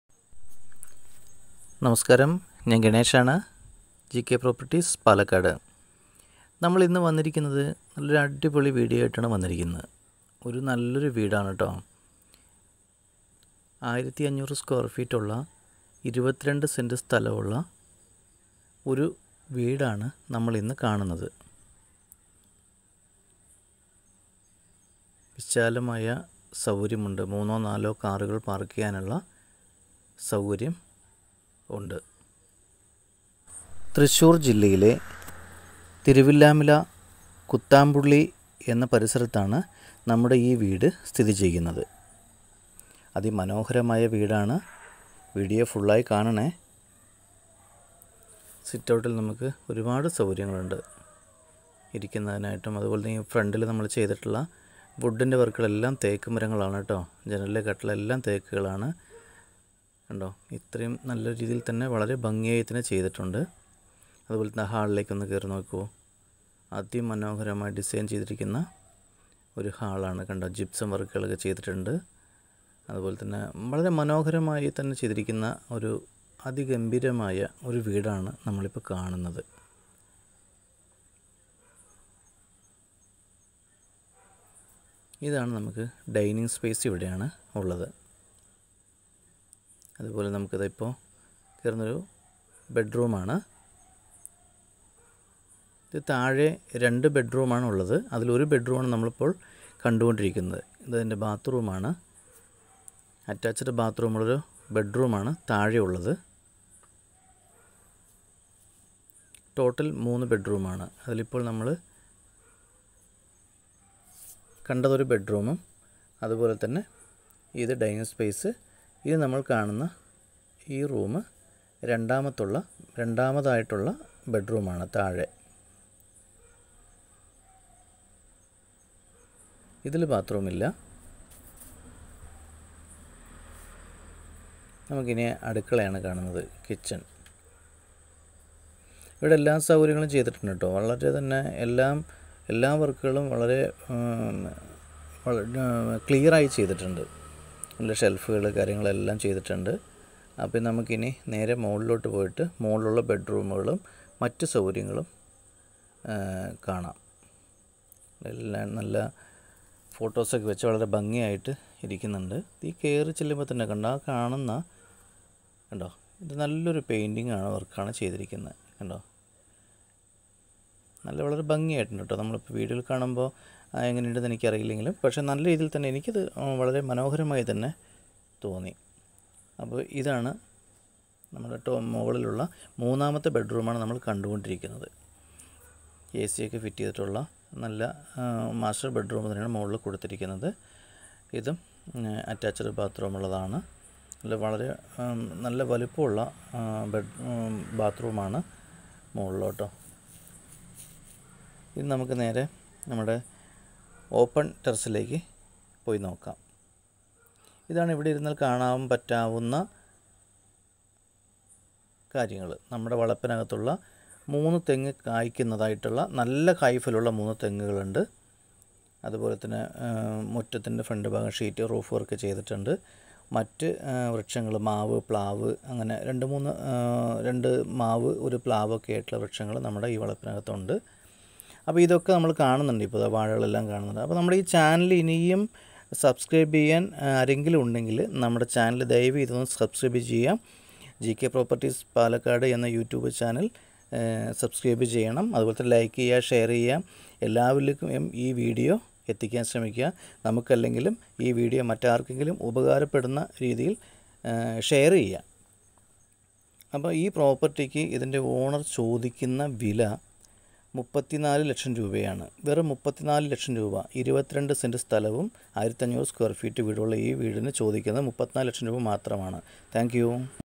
അർജന്റ് വില്പന 22 സെന്റ് സ്ഥലവും 1500 Ft വീടും സൂപ്പർ വീട് വെറും 34 ലക്ഷത്തിന് വിളിക്കു 9961366601
തൃശ്ശൂർ ജില്ലയിലെ തിരുവില്ലാമല കുത്താമ്പുള്ളി ഭാഗത്ത് 22 സെൻറ് സ്ഥലവും 1500 സ്ക്വയർ ഫീറ്റ് പുതിയ വീട് ബാങ്കിൽ ലോൺ ഉള്ള പ്രോപ്പർട്ടി ആണ് അതുകൊണ്ടാണ് അർജൻറീന വില്പന എന്ന് പറയുന്നത് മൂന്ന് ബെഡ്റൂം ഉള്ള വീടാണ് അതിൽ രണ്ട് ബെഡ്റൂം അറ്റാച്ച്ഡ് ബാത്റൂം ഉണ്ട് താഴെ നിലയിൽ രണ്ടു ബെഡ്റൂമും മുകൾനിലയിൽ ഒരു ഉള്ളത് തിരുവില്ലാമല യിലേക്ക് രണ്ട് കിലോമീറ്റർ ദൂരവും കുത്താൻ പള്ളിയിലേക്ക് ഒരു കിലോമീറ്റർ ദൂരവും മാത്രമാണ് ടൗൺ പ്രദേശമായതിനാൽ സ്കൂളുകളുടേയും ആരാദനാലയങ്ങളുടേയും മറ്റു സൗകര്യങ്ങളെയും പ്രത്യേകം പറയേണ്ടതില്ലല്ലോ? കൂടുതൽ വിവരങ്ങൾക്ക് ജികെ പ്രോപ്പർട്ടീസ് ഉമായി ബന്ധപ്പെടുക Ganesh,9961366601